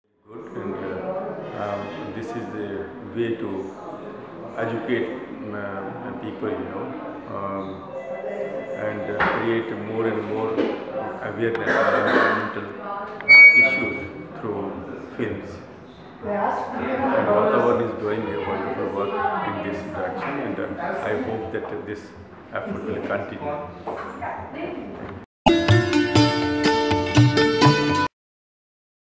Mr M C Mehta, Supreme Court Lawyer and Environmentalist
A lawyer by profession and a committed environmentalist by choice, M C Mehta has made the fight to protect India's environment his unending mission. He has pioneered legal activism for environmental protection and is proof that one man can make a difference. Award Jury Members (2015)